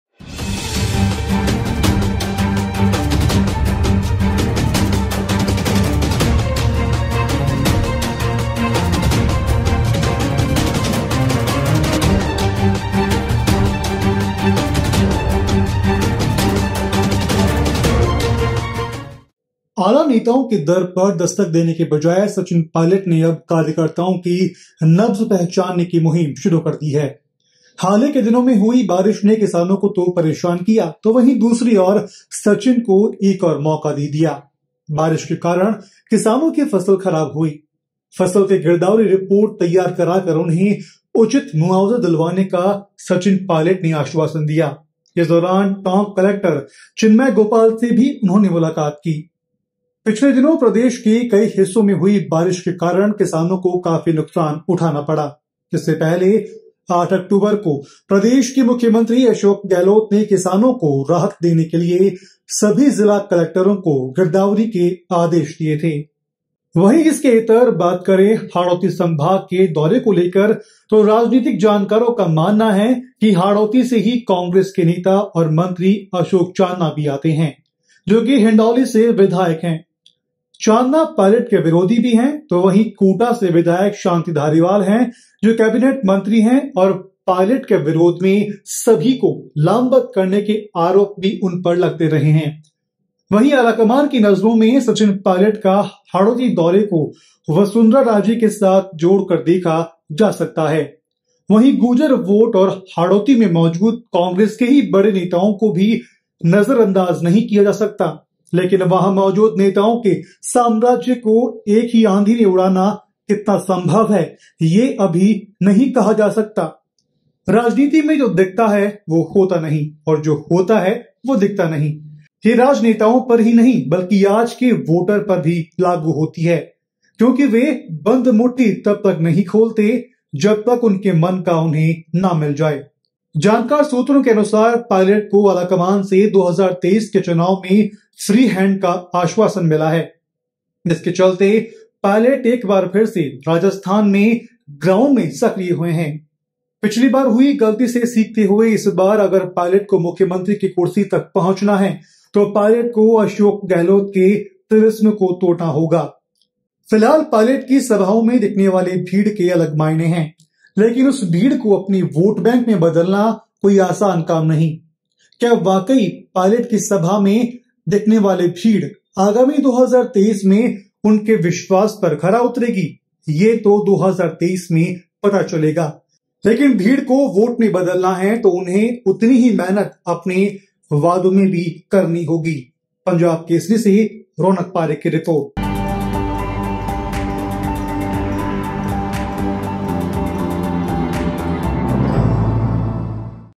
क्या है पायलट की रणनीति ? जानिए रिपोर्ट में | Sachin Pilot Plan | Rajasthan Congress
आला नेताओं के दर पर दस्तक देने के बजाए सचिन पायलट ने अब कार्यकर्ताओं की नब्ज पहचानने की मुहिम शुरू की है. हाल ही के दिनों में हुई बारिश ने किसानों को तो परेशान किया है वहीं दूसरी और सचिन को एक मौका और दे दिया. बारिश के कारण किसानों की खराब हुई फसल की गिरदावरी रिपोर्ट तैयार कराकर उन्हे उचित मुआवजा दिलवाने का आश्वासन दिया.